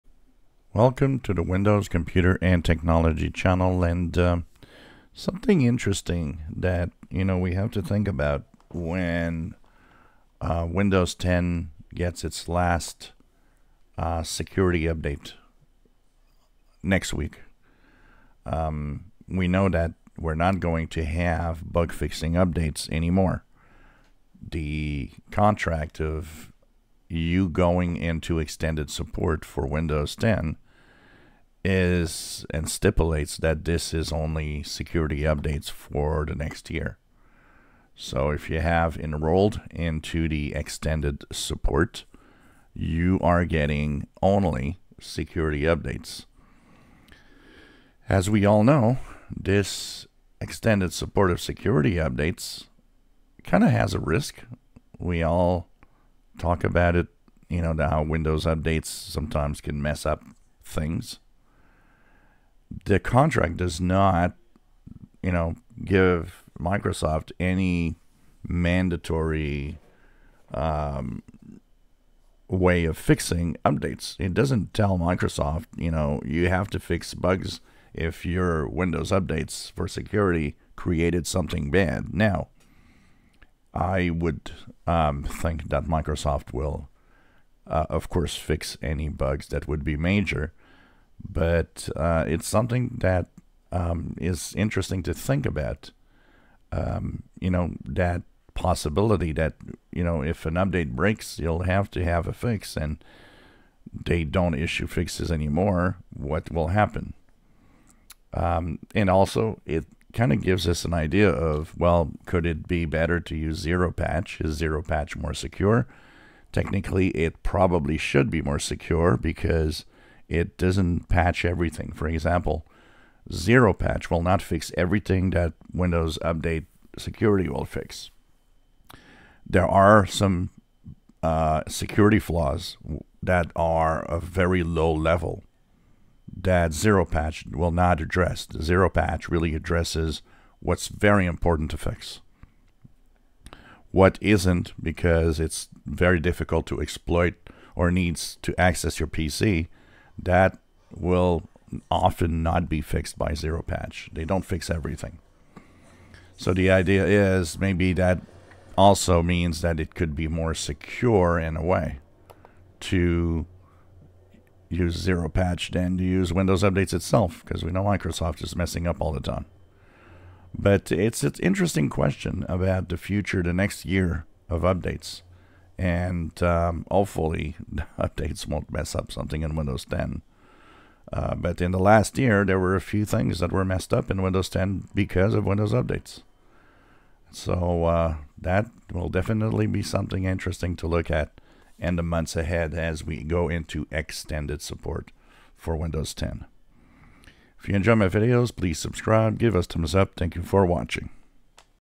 Windows 10 Extended support does raise a question about no more bug fixes
What if a update breaks Windows 10 .